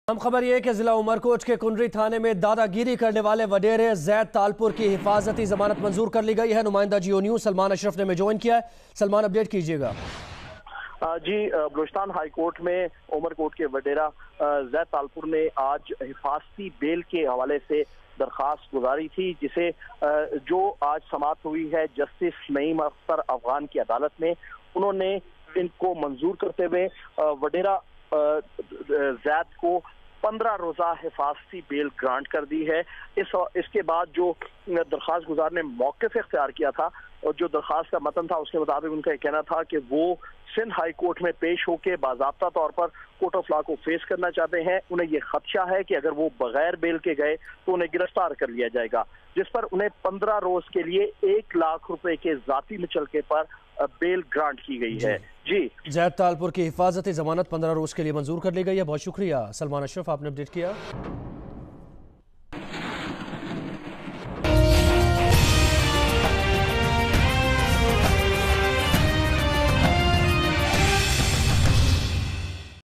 Balochistan HC approves Zaid Talpur’s petition for protective bail 08-May-2017
Balochistan HC approves Zaid Talpur’s petition for protective bail